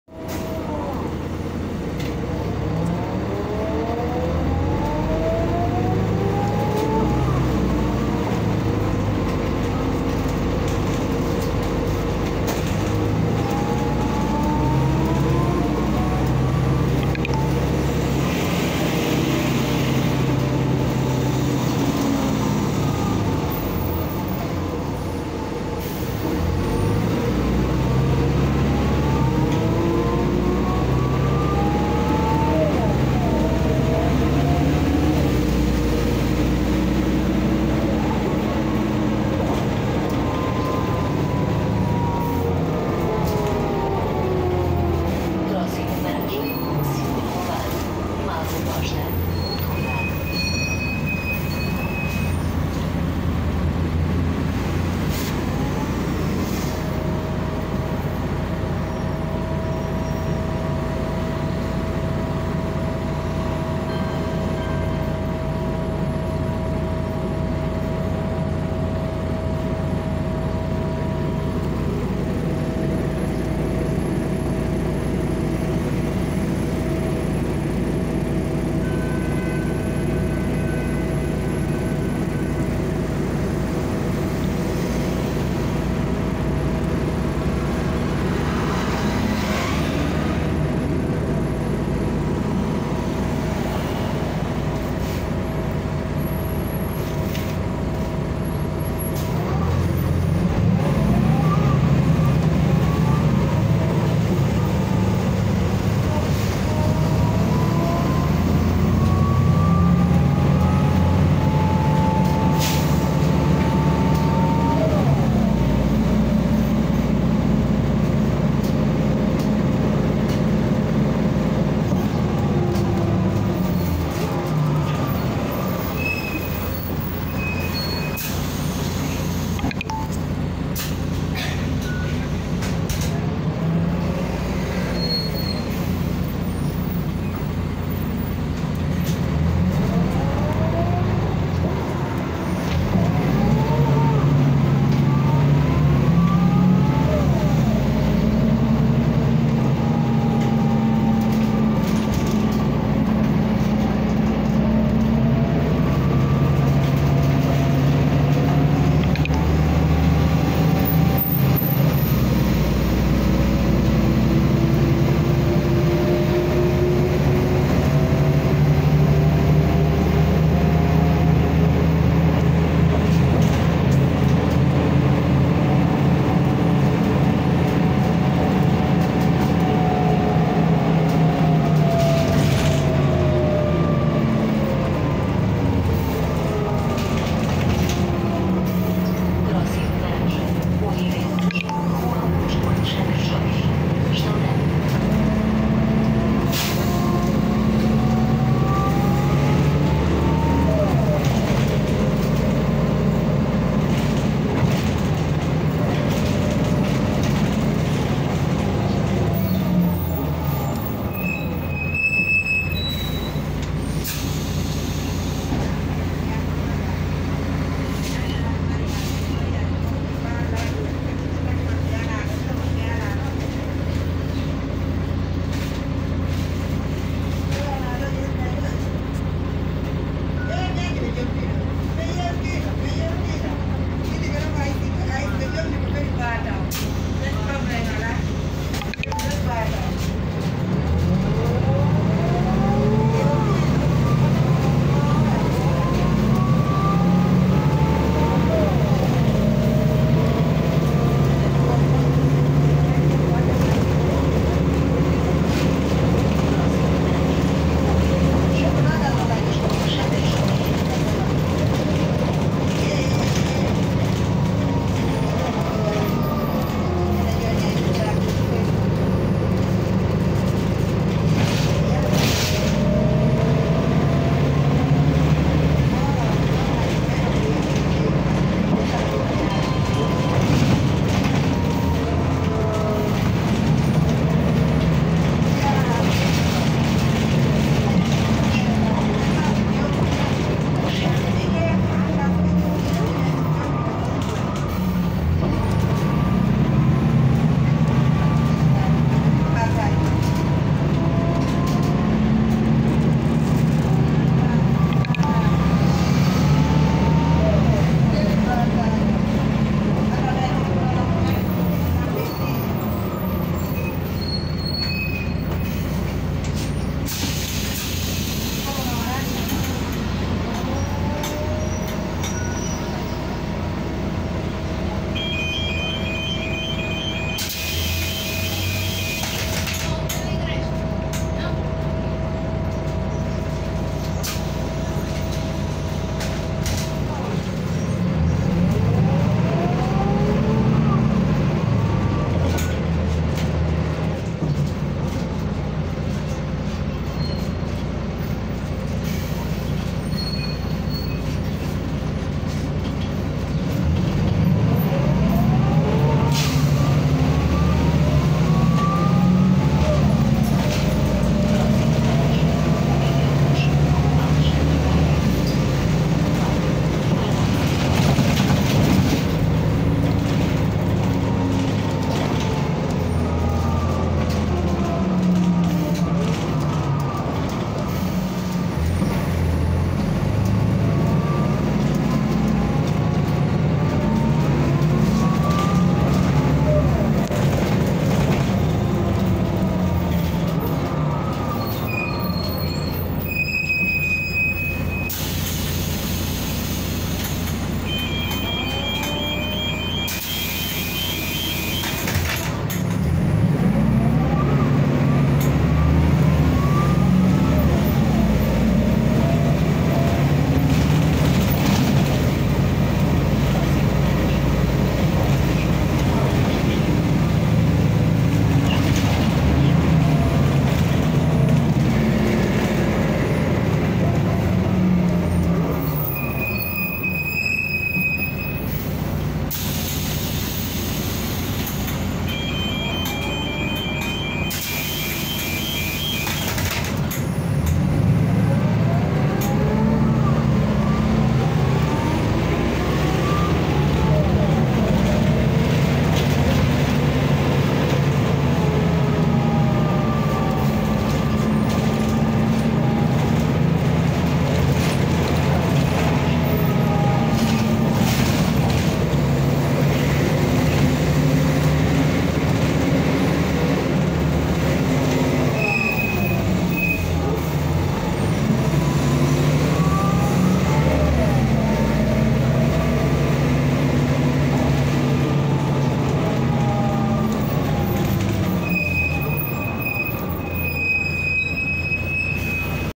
A bordo de RL 152
RL 152
Chassis:Mercedes-Benz O405N2
Carroçaria:Camo Camus
Motor:Mercedes-Benz OM447hLA (299cv)
Caixa de velocidades:ZF 4HP 500 (4 speed)
Ano:1998
Carreira:216